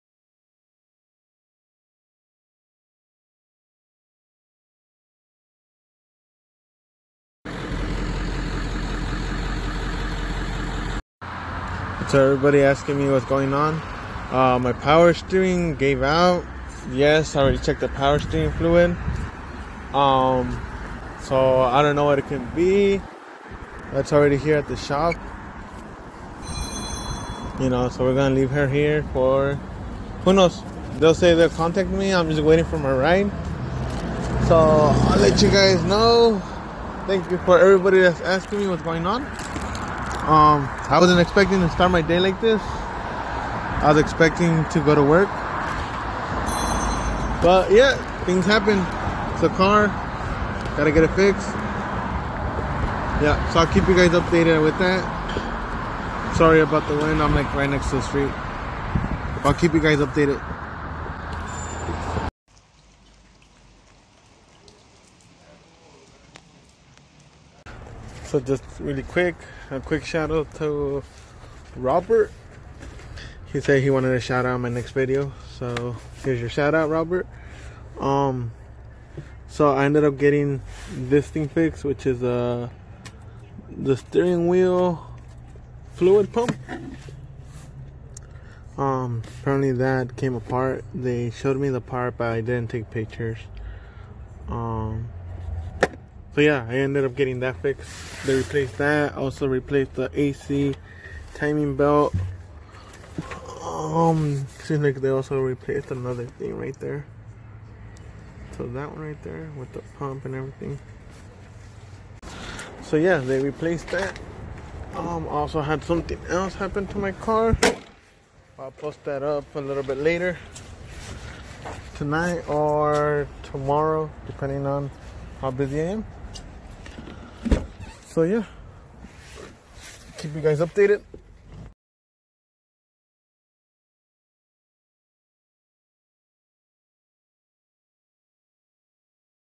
Steering pump gave out!!!
My steering gave out on the YUKON DO IT missed a day of work. Thanks to the guys at the Allen Tire company for coming thru. Also thanks for letting me know about the front driver side tire.